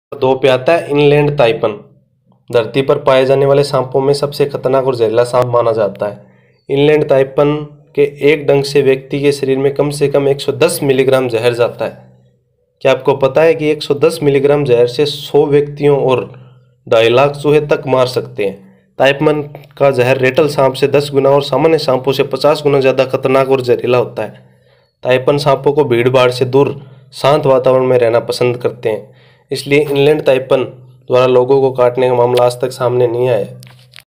दुनिया के 10 सबसे जहरीले साँप (part -9)top10
दुनिया के 10 सबसे खतरनाक सांप ।
इस वीडियो में चर्चा करेंगे इनके बारे में ही।
इनलेणृड ताईपन जो धरती पर पाये जाने वाला सबसे ज़हरीला साँप हैं।